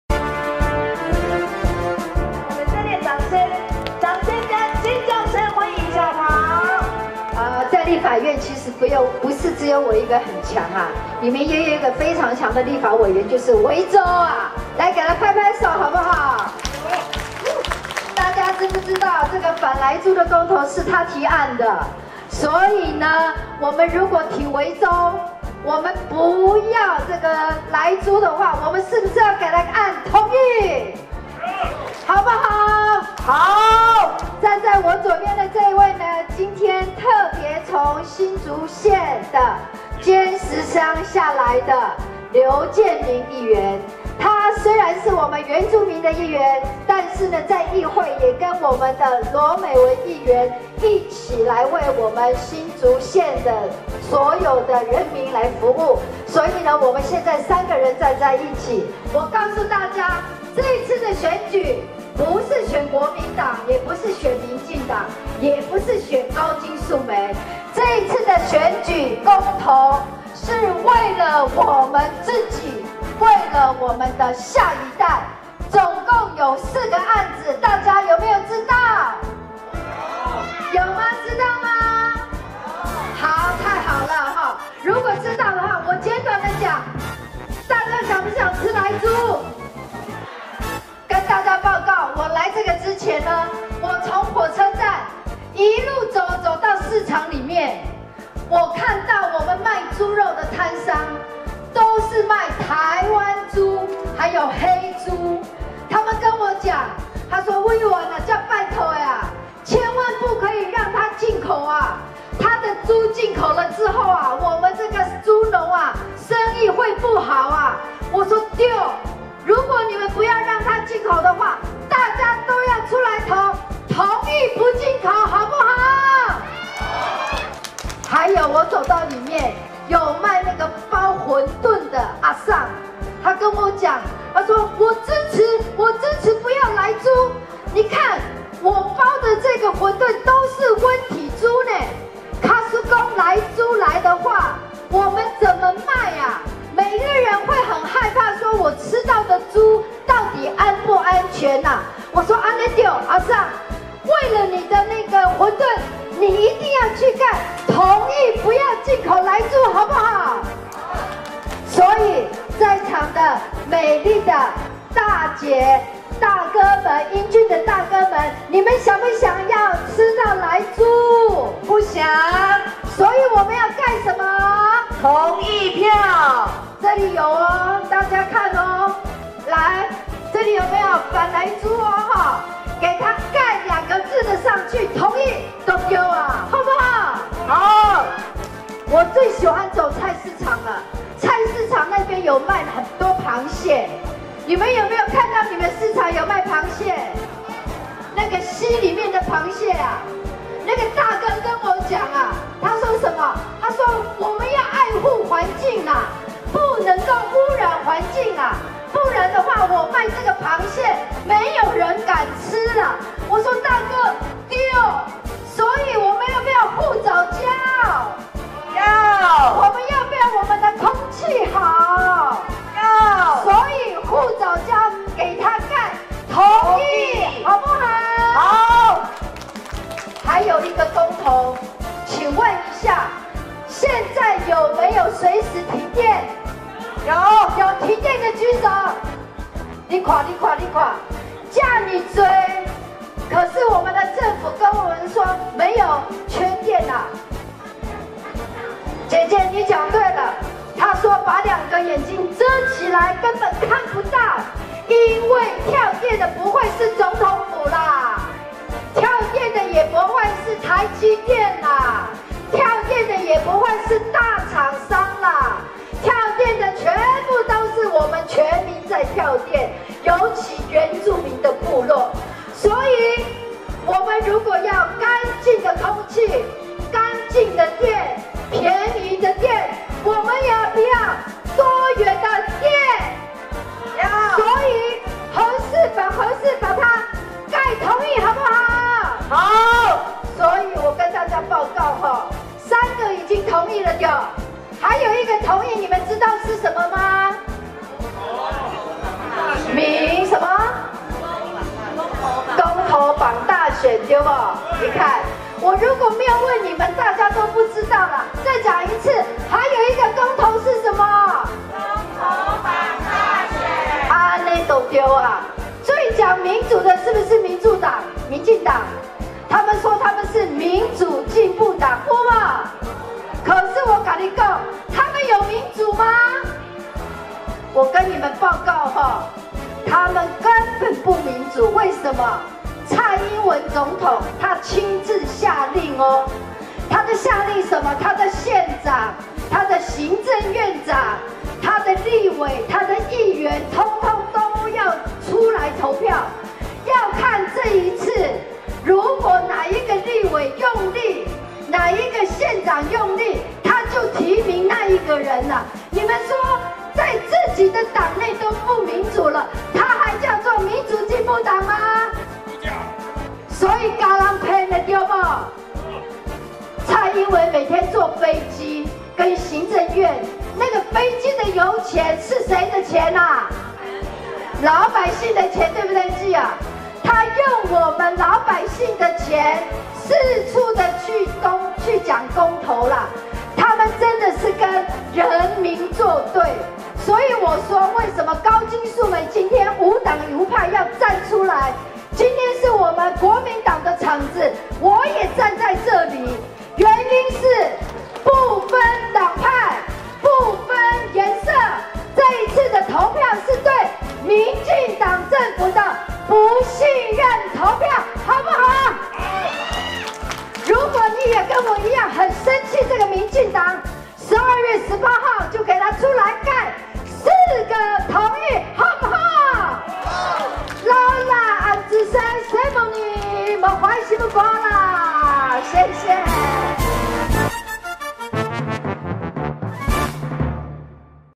《湖口媽祖廟 公投宣講》～高金素梅2021.12.04
《湖口媽祖廟 公投宣講》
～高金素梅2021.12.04

今天上午來到湖口媽祖廟，參加國民黨新竹縣黨部的公投宣講場。